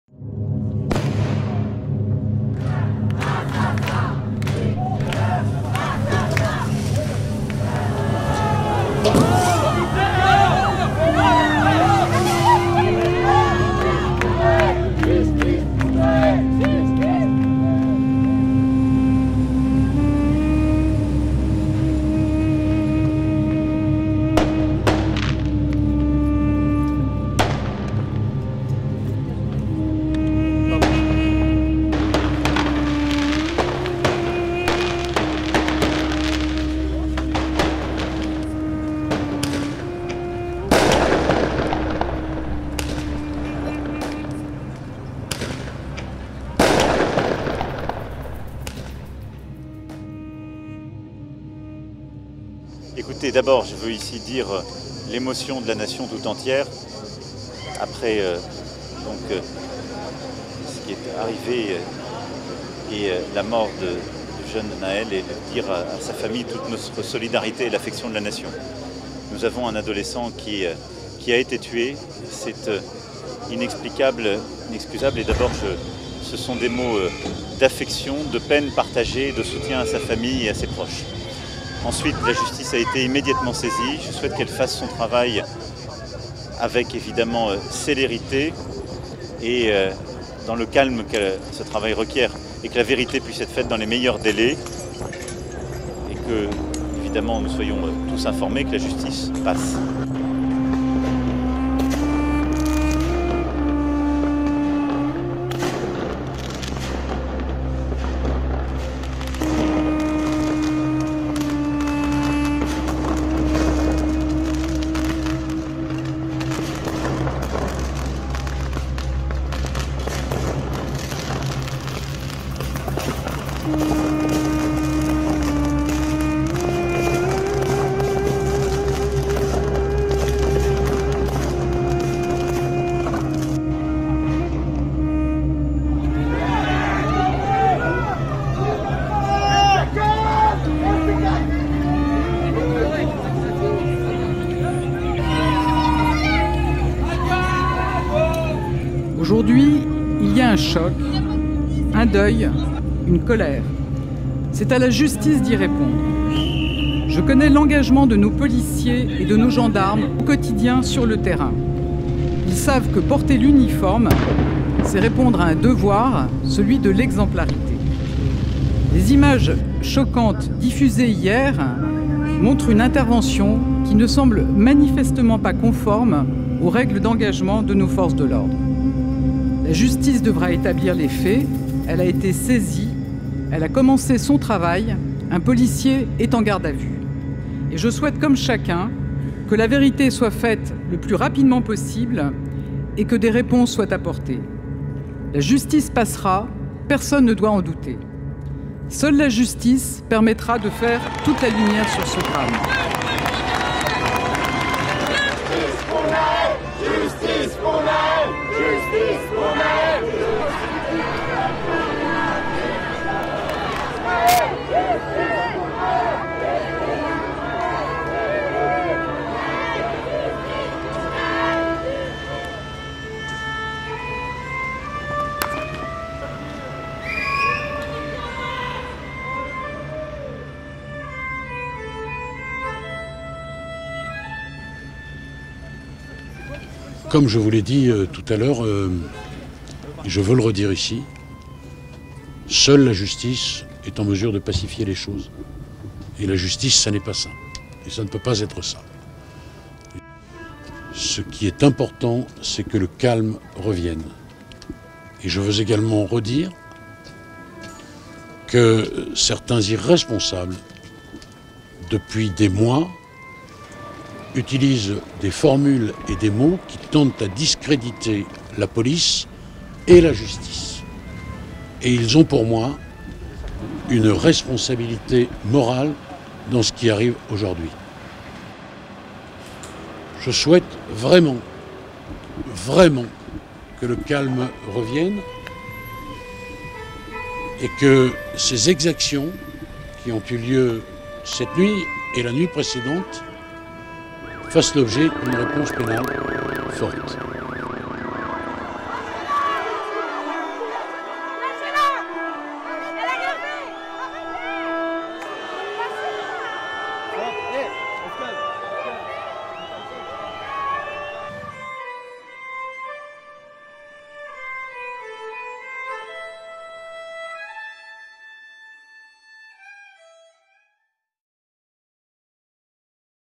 Morte de adolescente pela polícia detona revolta em Paris
A reação à morte de um adolescente de origem argelina de 17 anos pela polícia de Paris, na terça-feira (27), foi o gatilho para uma onda de protestos nas ruas da cidade, com enfrentamentos entre policiais e manifestantes e dezenas de veículos incendiados.